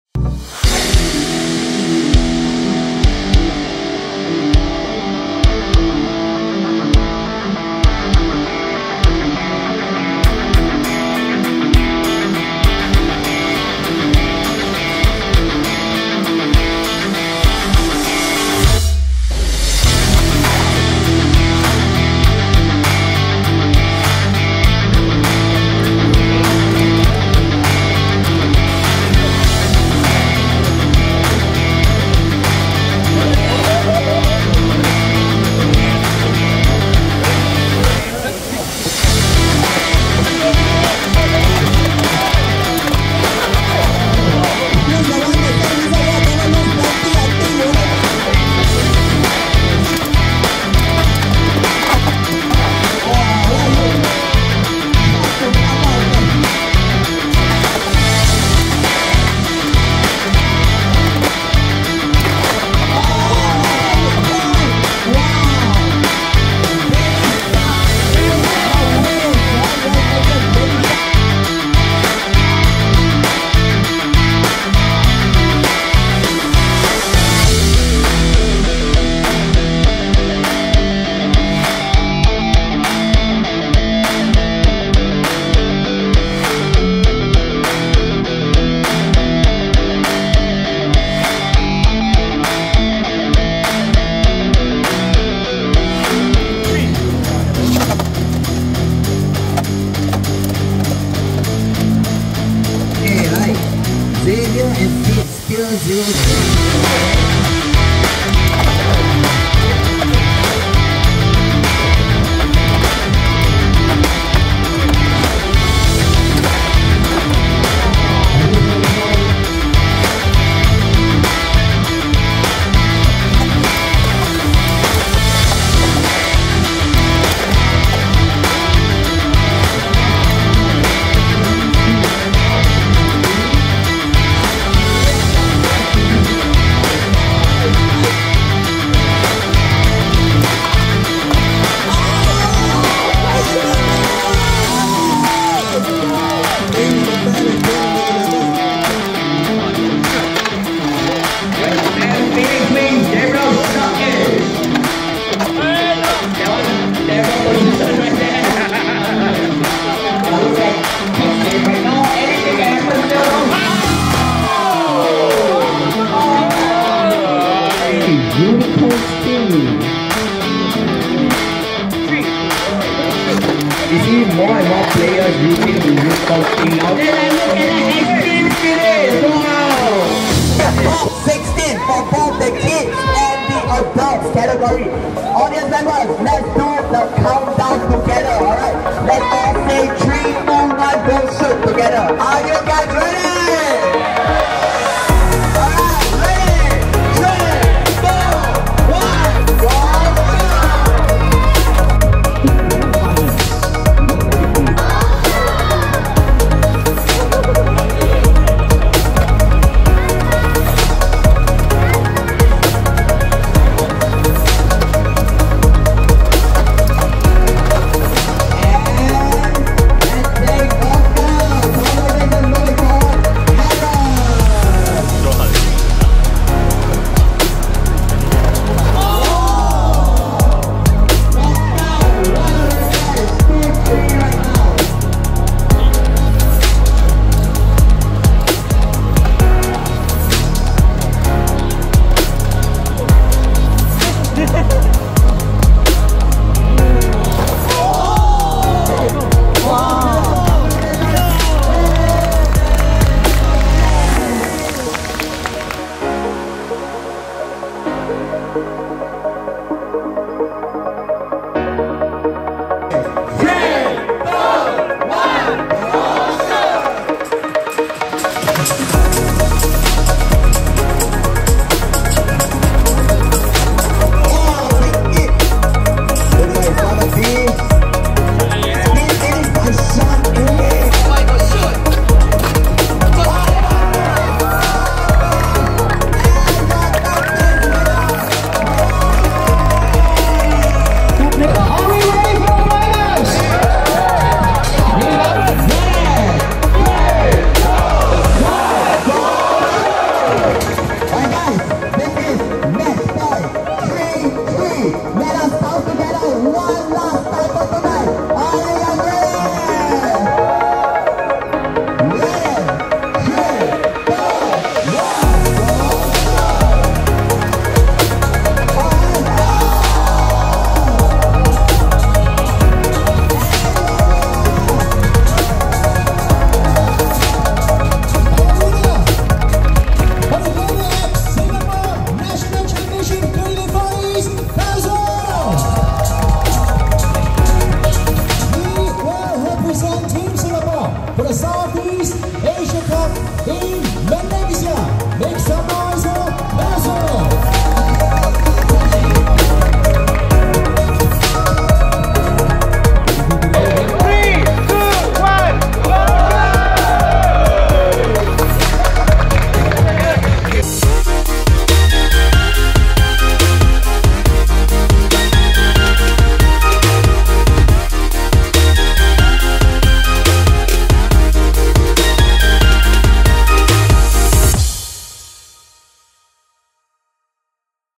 【公式G1大会】BEYBLADE X G1 X-TREME CUP NATIONAL CHAMPIONSHIP SINGAPORE 2024 HIGHLIGHTS | BUGIS+『ベイブレードx』
Experience the first Beyblade X Singapore National Championship 2024!  Step into the vibrant Bugis+ Level 2 Atrium from May 13th to 19th and immerse yourself in the epic showdown of skills, strategies, and spinning tops as the nation's top Bladers compete for glory where the winners will be representing Singapore at the Sea Cup in Kuala Lumpur, Malaysia in July.

Winners Combo:
🥇 Kazu (1st place):
SE 1-60 F
PW 3-60 R
WR 5-60 H

🥈Fiz (2nd place):
DS 9-60 H
PW 9-80 T
WR 1-60 R

🥉Byran (3rd Place):
PW 3-60 R
TB 5-60 O
WR 9-60 B

🎖️Gabriel - Beyblade Genki (4th Place):
SE 3-60 LF
US 1-60 H
WR 9-60 B

Chapter:
00:00 - Introduction
00:19 - Time Attack Challenge
00:25 - G3 Tournament (13 - 16 May)
00:32 - G3 Team Battle (17 May)
01:10 - G1 Qualifying Sessions (18 - 19 May)
01:36 - G1 Qualifying Session (Junior Cat)
01:59 - G1 Qualifying Session (Open Cat)
03:00 - G1 Semi-Finals
04:23 - G1 Finals (3rd Place Battle)
04:51 - G1 Finals (1st & 2nd Place Battle)